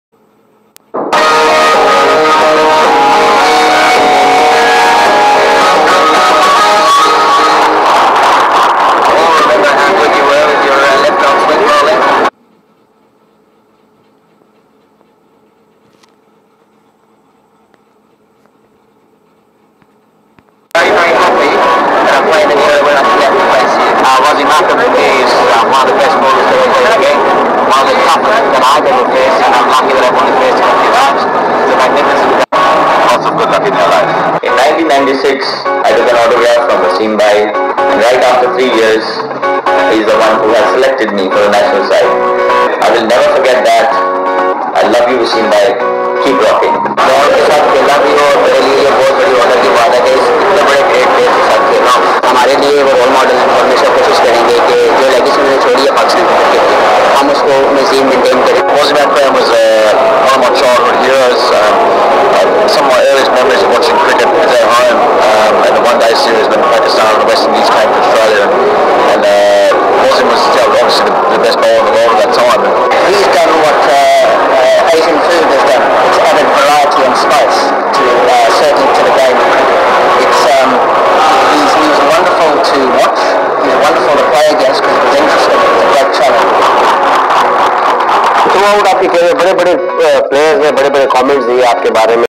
Shoaib malik pakistani Sportsman
Shoaib malik and other sports men are admiring Waseem akram a former  sports man of pakistan